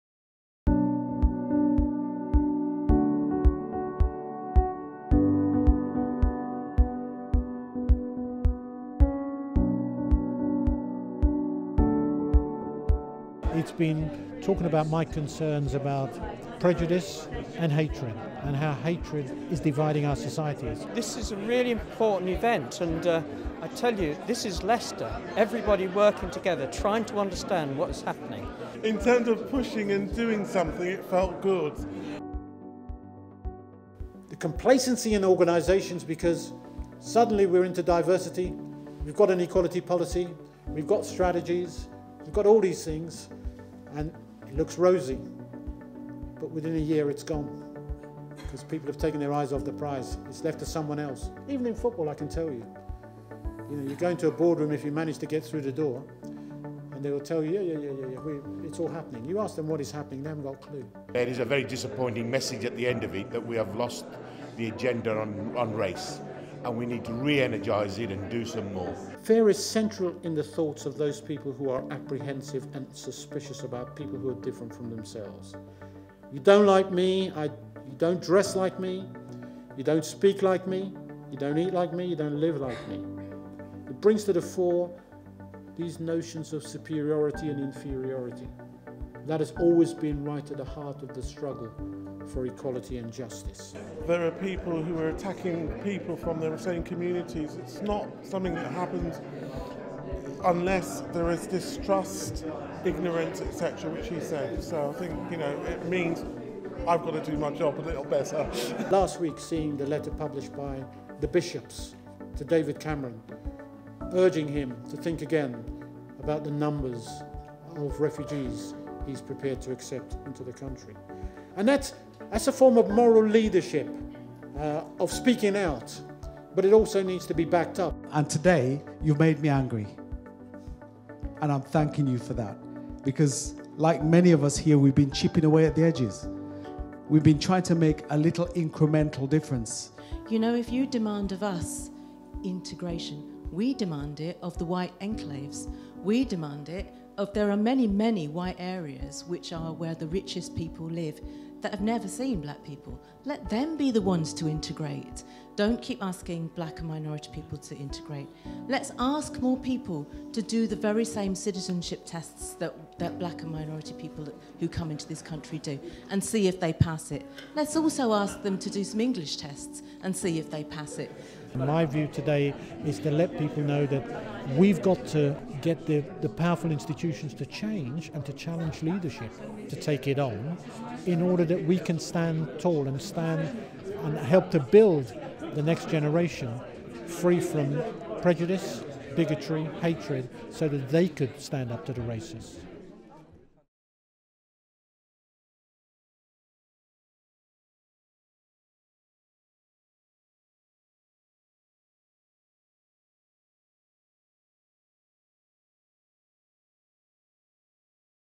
DICE launch event
On October 22nd 2015 the new Centre for Diversity, Inclusion and Community Engagement (DICE) at the University of Leicester was officially launched before an audience in excess of 350 people.

A presentation on Living in a Diverse Britain: Aspiration, Fears and Different Realities was delivered by the former Chair of the Commission for Racial Equality and member of the House of Lords, Lord Herman Ouseley.

Here is a short film about the event.